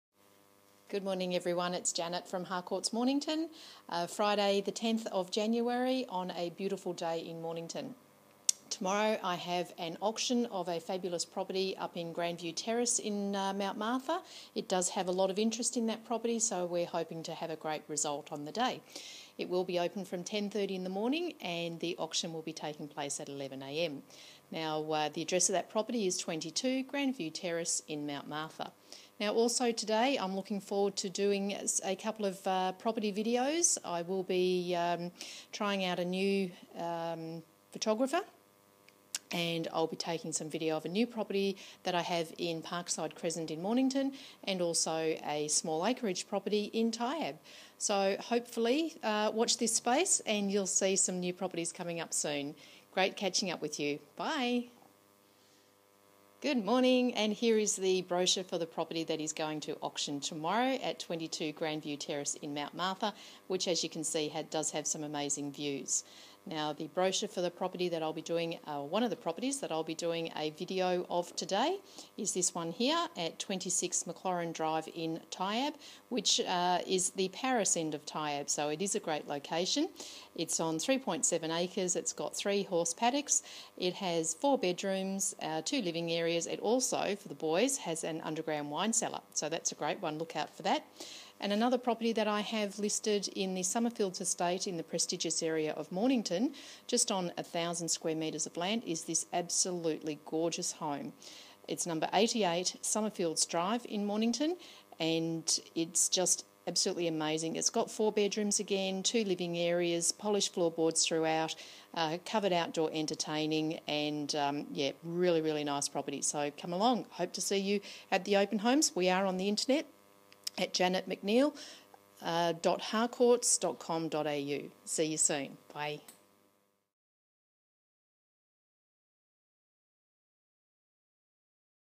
Janet McNeill - Your Auction Specialist
Janet McNeill is your local Auction specialist. Janet is confident of achieving excellent results for her vendor clients, with minimum fuss and no stress. Be sure to contact her if you would like further details.

Janet McNeill
Licensed Estate Agent
Harcourts Mornington
Suite 2/1A Main Street, Mornington Vic 3931
AUSTRALIA